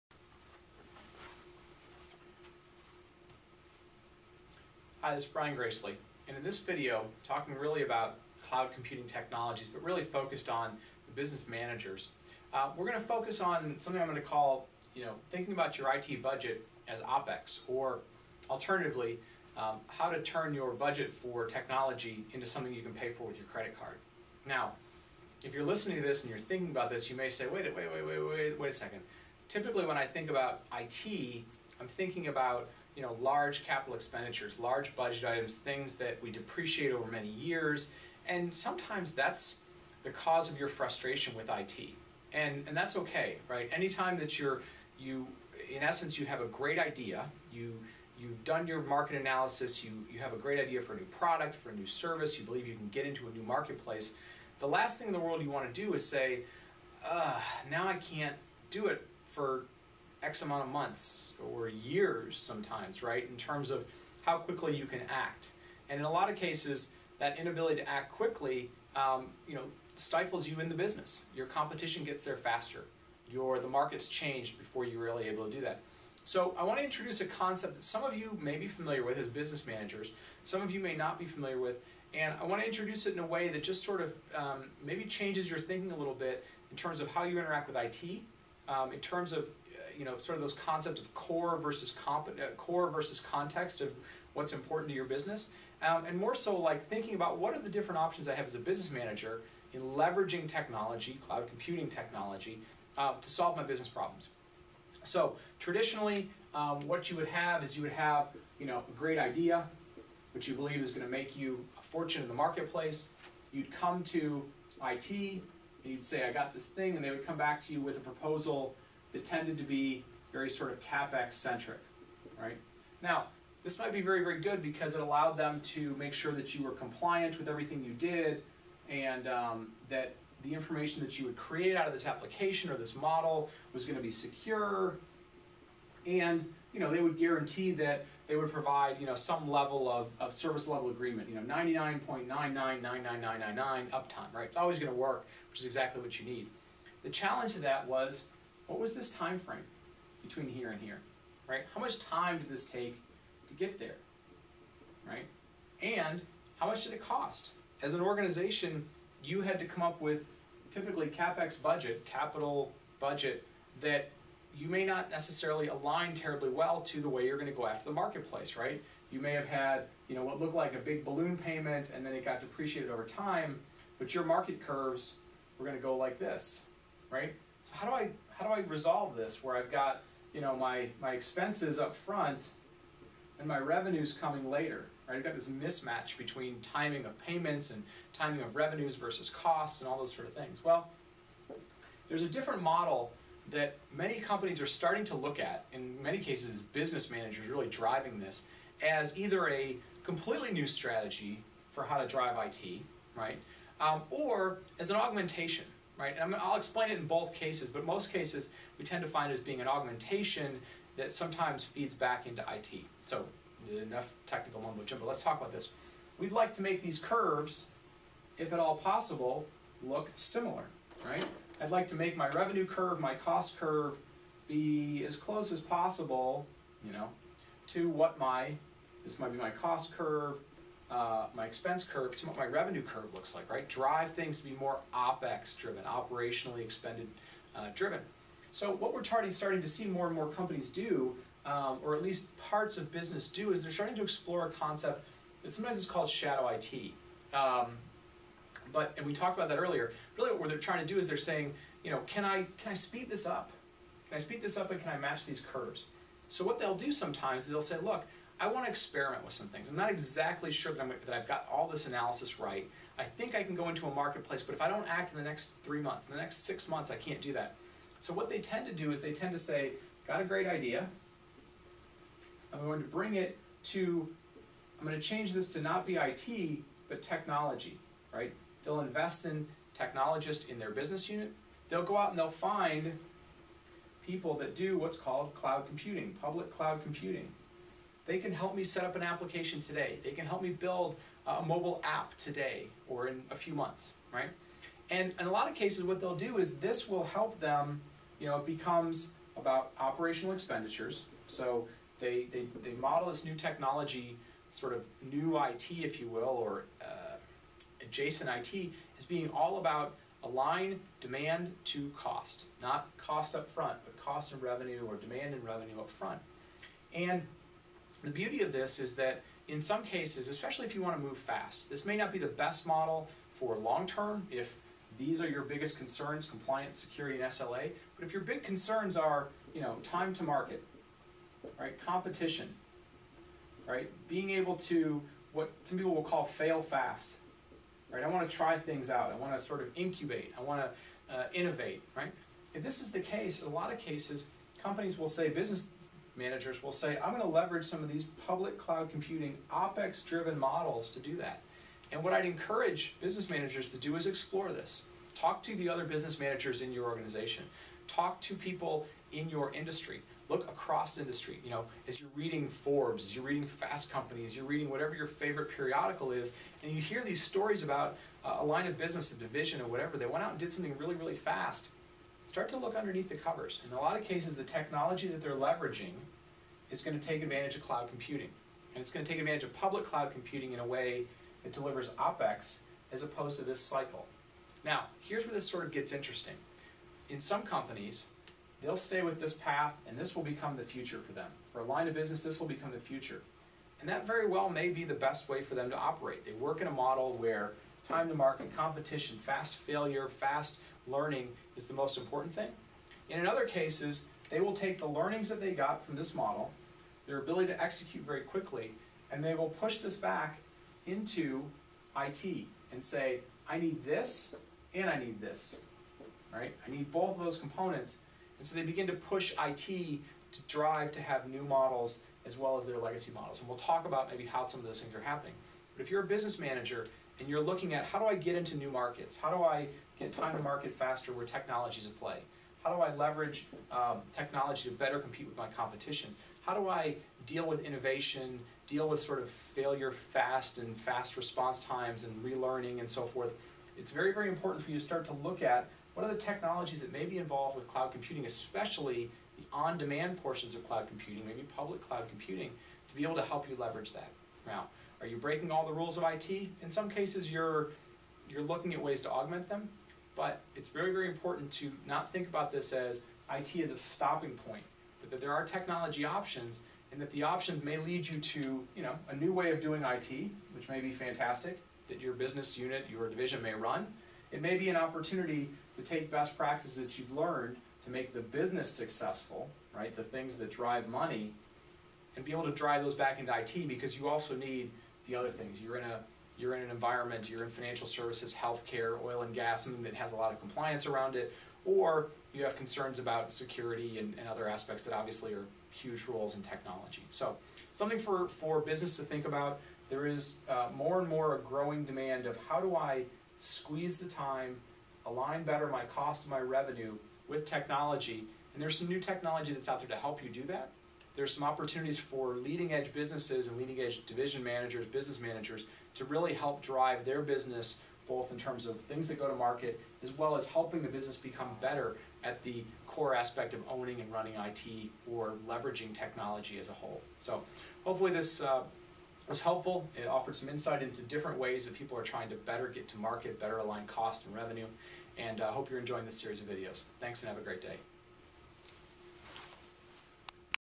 The Cloudcast - Cloud Computing for Business Managers - Expensing Your IT Budget on a Credit Card
Brian Gracely discusses how Business Managers can increase their business opportunities by leveraging on-demand Cloud Computing resources.